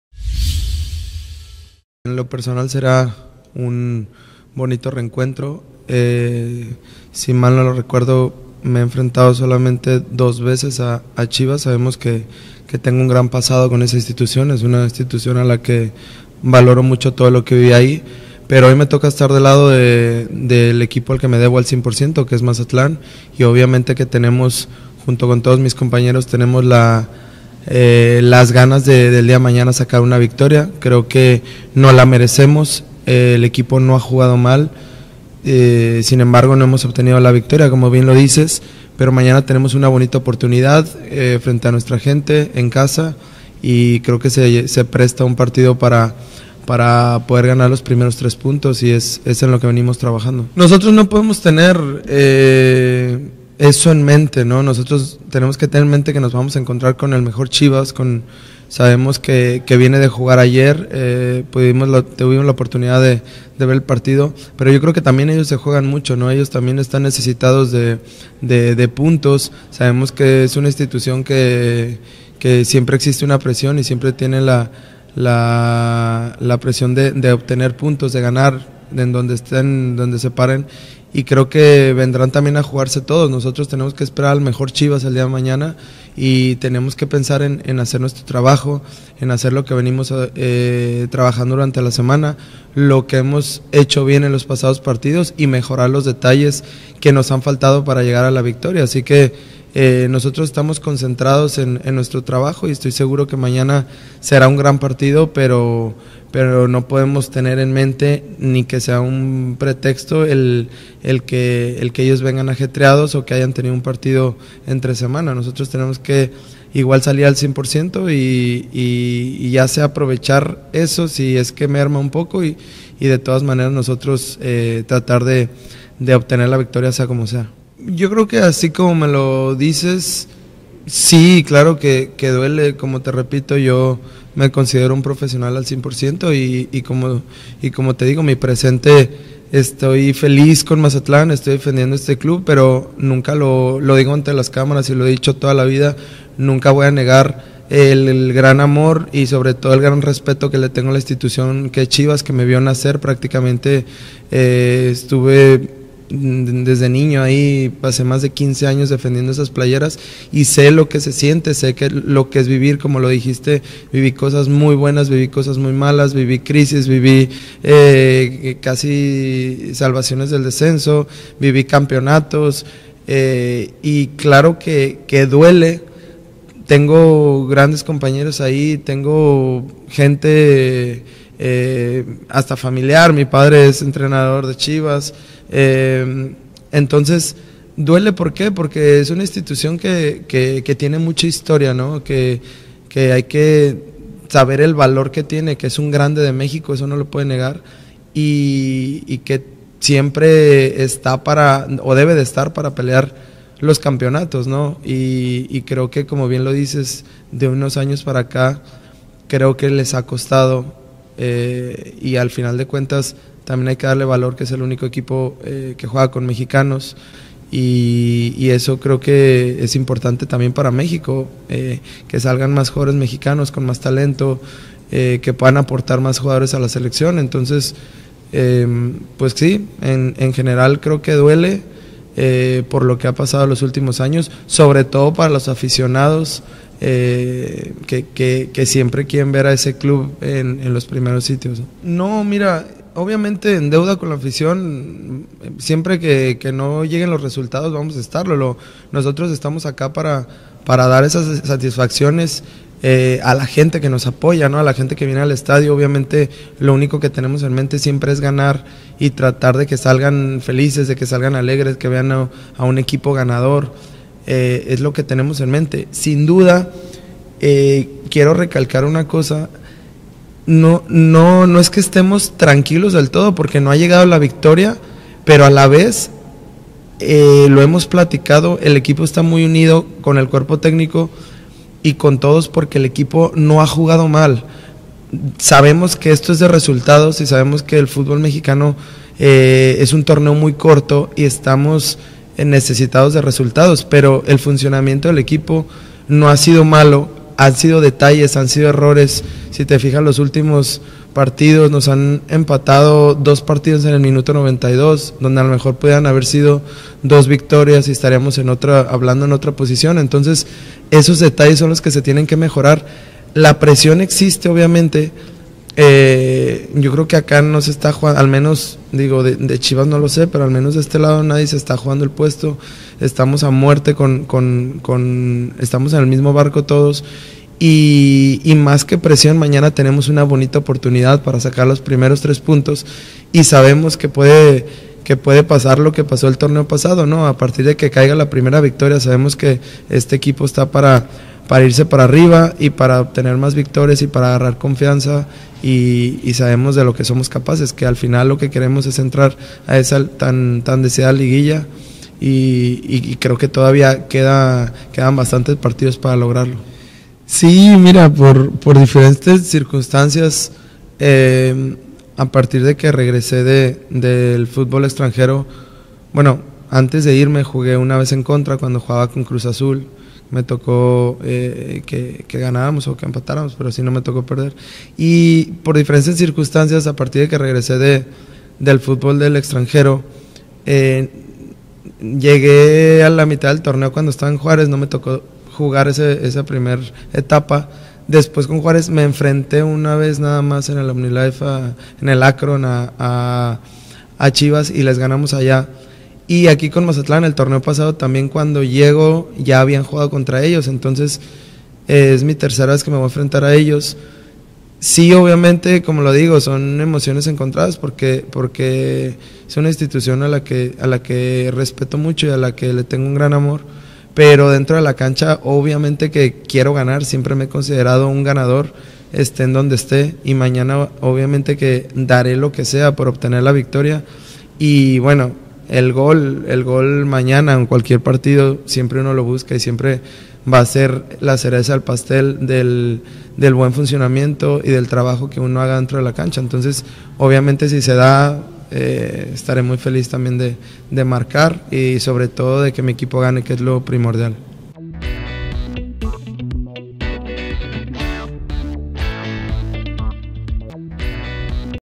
Olvida Marco Fabián su amor por Chivas
El futbolista de Mazatlán aceptó que Guadalajara es su "primer amor", sin embargo, dentro de la cancha lo olvida y sólo piensa en que los Cañoneros ganen.